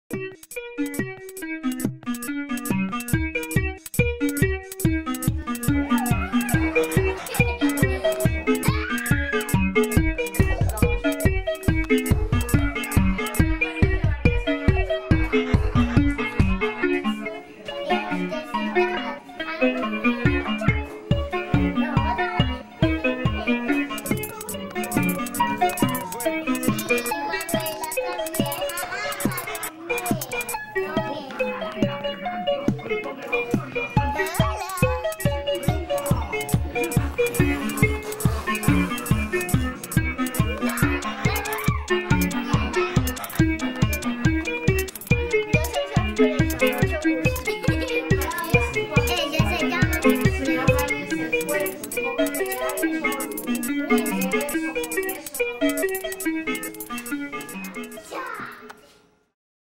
Videoensayo - TV infantil
Los niños tienen distintas maneras de jugar y aunque en nuestra casa creativa, muchos de los que allí trabajan creen que no han crecido, hoy no servían para nuestro video Ensayo.

Es por eso que uno de los integrantes de nuestro equipo de cosas locas, se fue a pasar una tarde con sus sobrinas, jugando un rato con ellas y viendo cómo se divierten los niños hoy en día. Veamos.